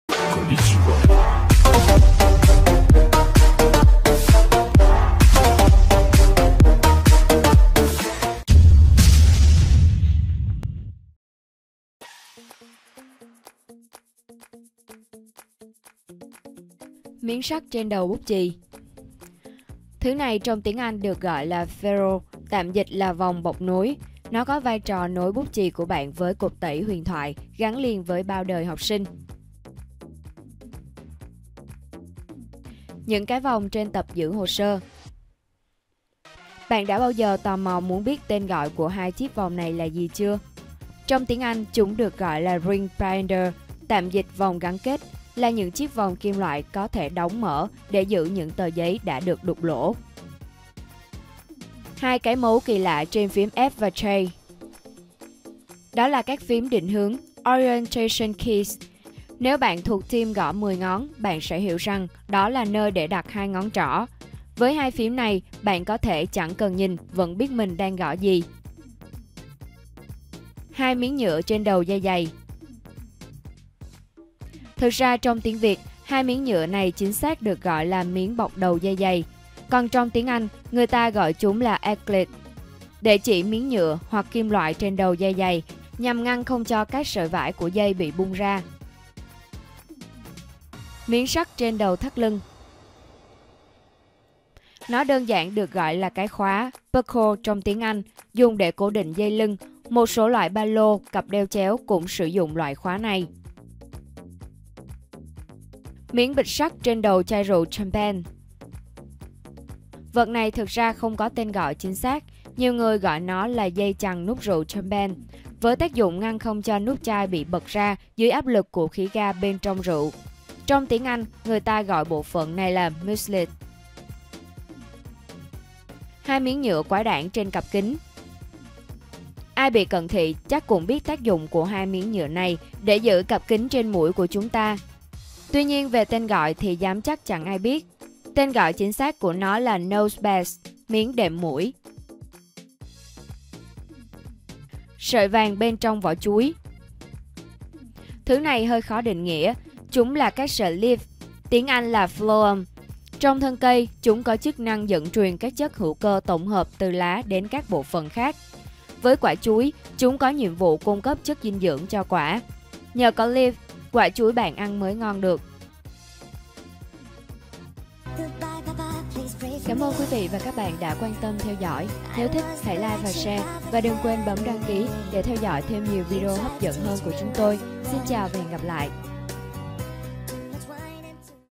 8 vật dụng lúc nào cũng nhìn thấy mà chẳng ai biết gọi tên ra sao
Cuộc sống quanh ta vẫn tràn ngập bí ẩn. Có những vật dụng nhìn rất quen, nhưng chẳng ai biết chúng là gì cả.